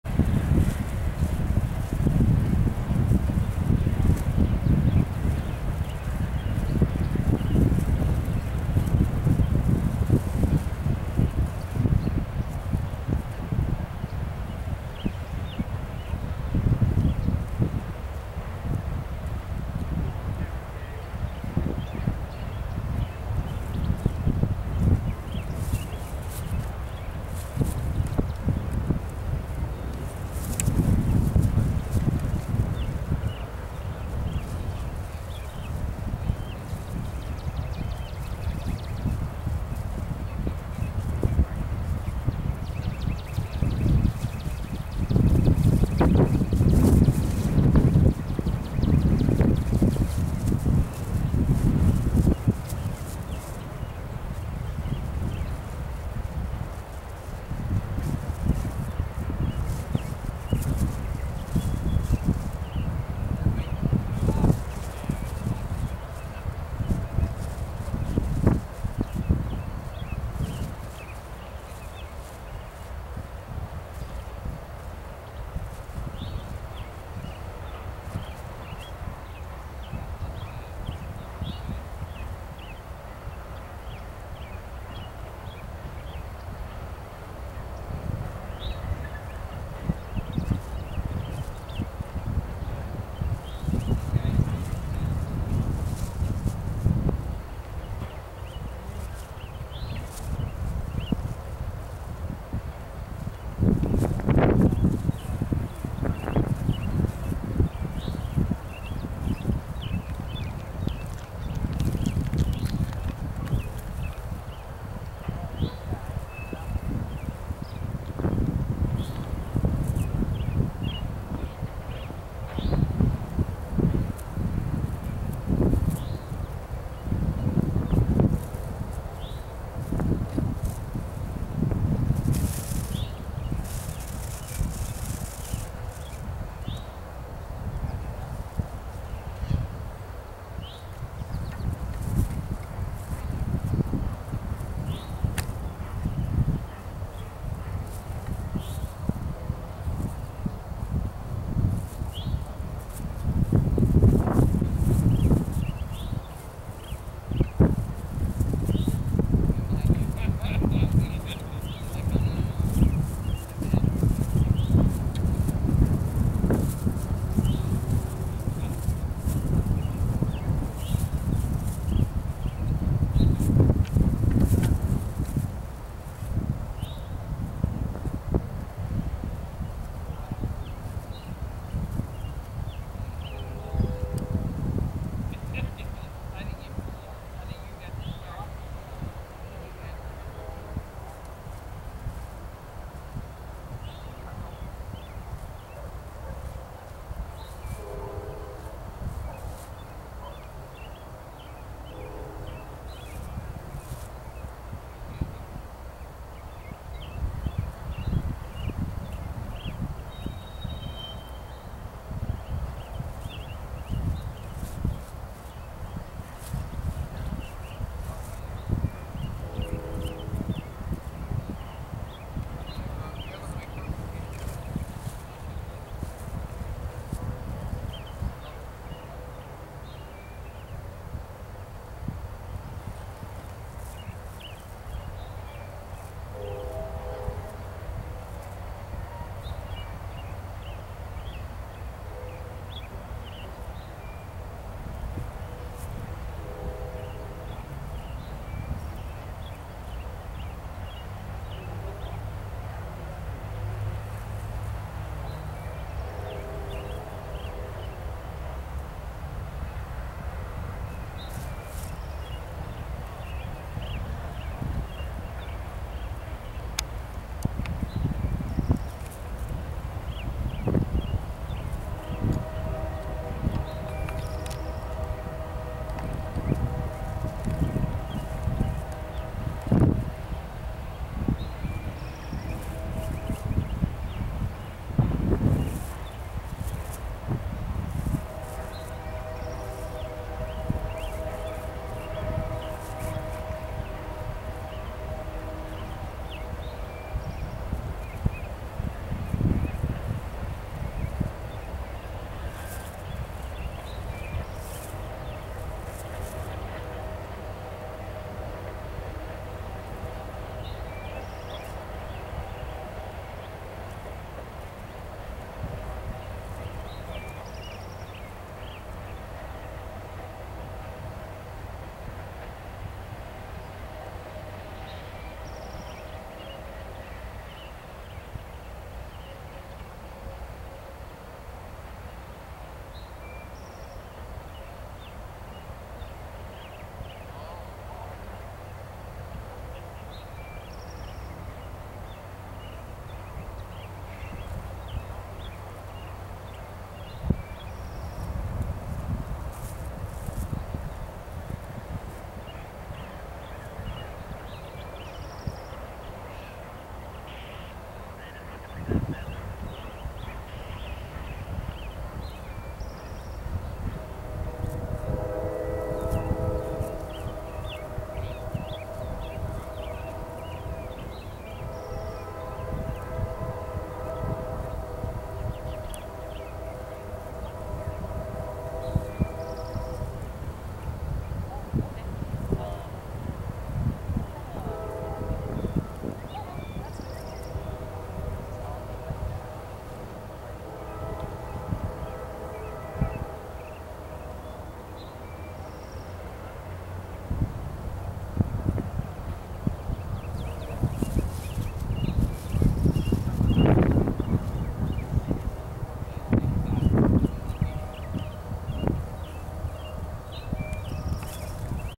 NS with UP in tripple header
a freight consist while  with three Union Pacific Diesels while waiting for the N&W j class 611 to do a photo run-by